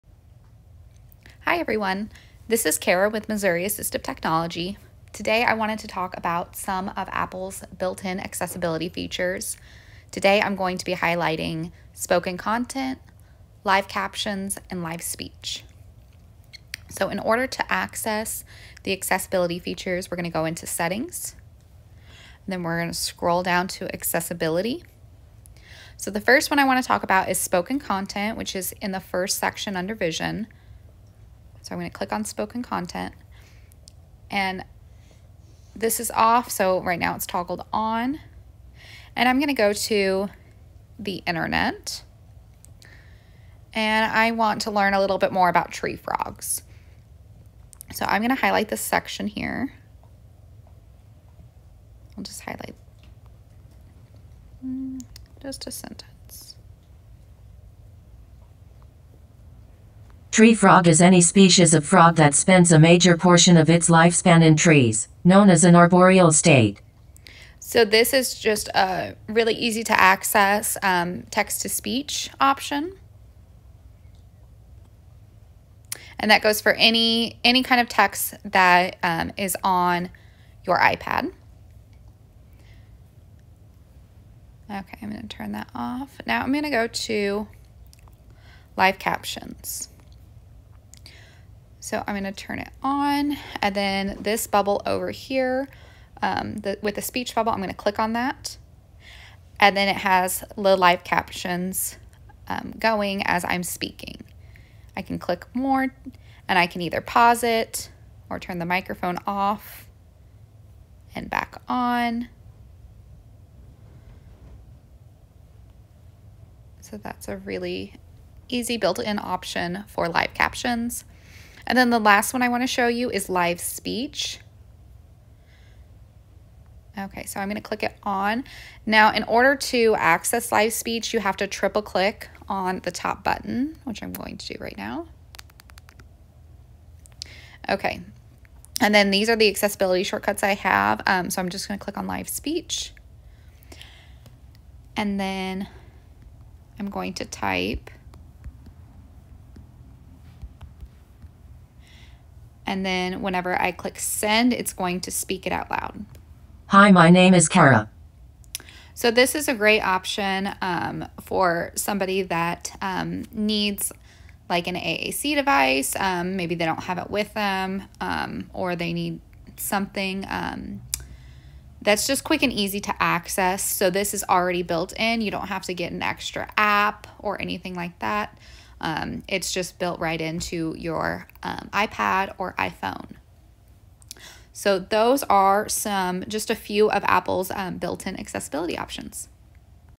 Built-In Apple Accessibility Features- Spoken Content, Live Caption, & Live Speech
This video briefly demonstrates some built-in Apple accessibility features like Spoken Content, Live Captions, and Live Speech